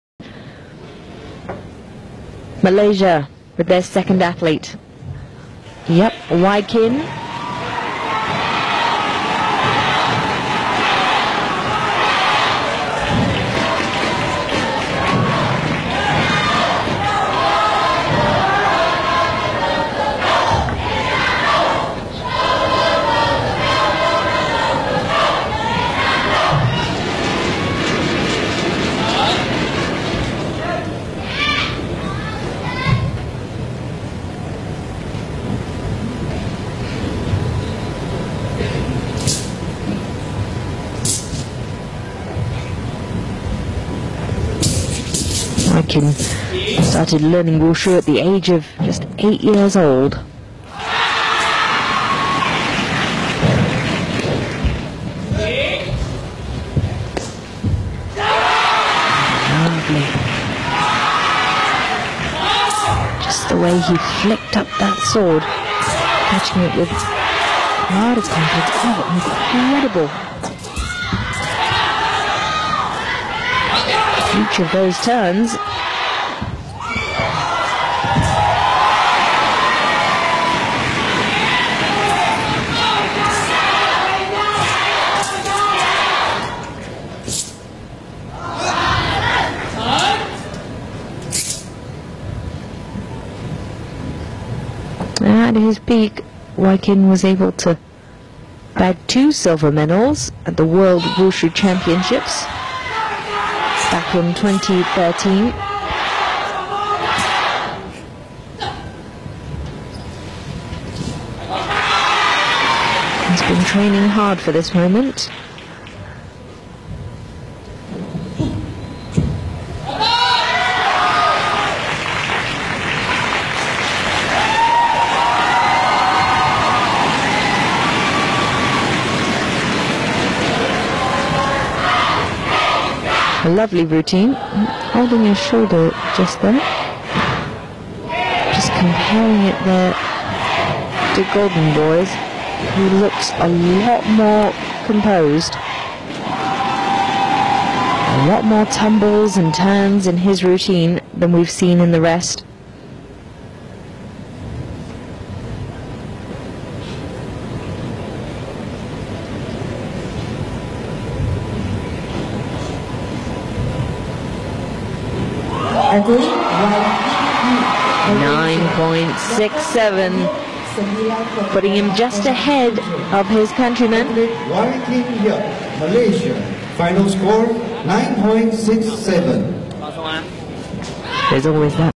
14 ЧМ по спортивному ушу 2017 |SEA GAMES 2017| Men's Jianshu | Yeap Wai Kin 🇲🇾 🇲🇾
Men's Jianshuнабрал 9,67 очков, чтобы выиграть золотую медаль.